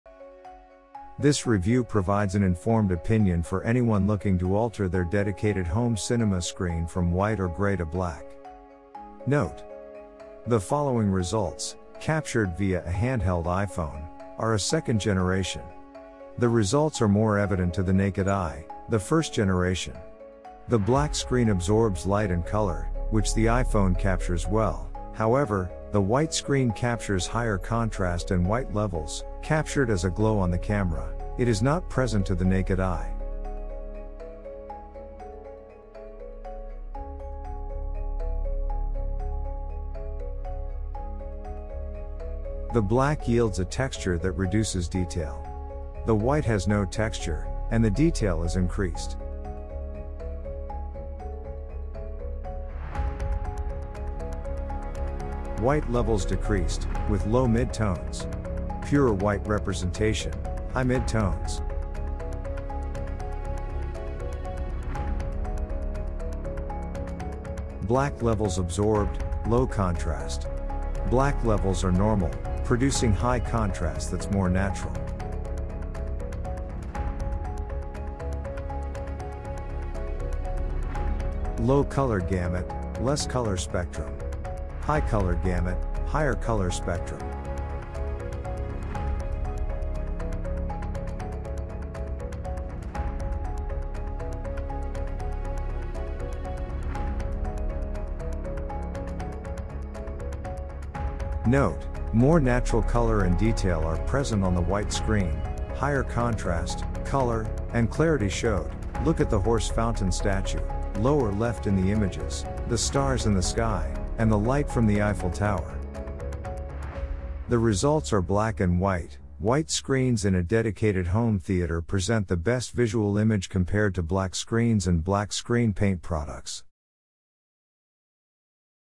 A BLACK & WHITE REVIEW: Extreme Black Widow, Crystal Edge Technology Vs White Screen - Home Theatre
This review provides an informed opinion for anyone looking to alter their dedicated home theatre screen from white or grey to black.

NOTE: Results, captured via a hand-held iPhone are a second generation. The results are more evident to the naked eye, the first generation.
The black screen absorbs light and colour, which the iPhone captures well; however, the white screen captures higher contrast and white levels, captured as a glow on the camera; it is not present to the naked eye.

RESULT: The results are black and white; white screens in a dedicated home theatre present the best visual image compared to black screens and black screen paint products.